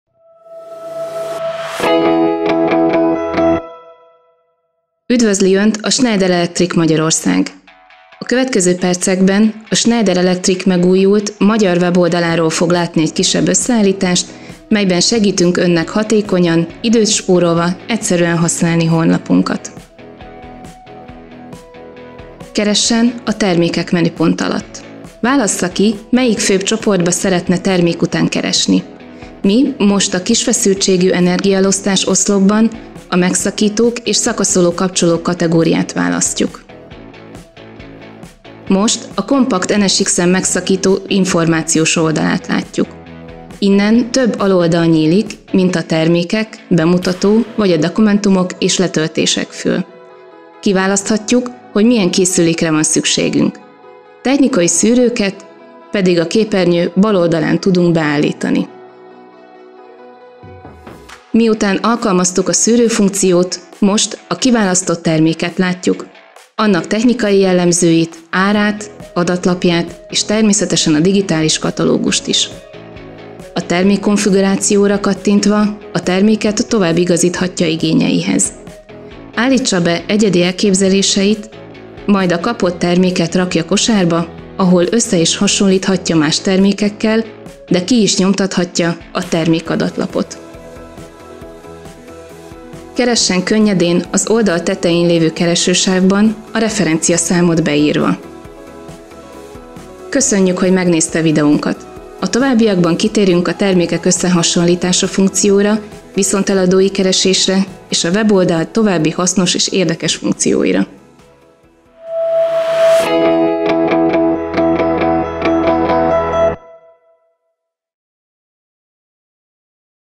Schneider Electric weboldal - oktató videó
Egy pár perces videó segítségével mutatjuk be a Schneider Electric megújult weblapját, és annak egyszerű, gyors használatát.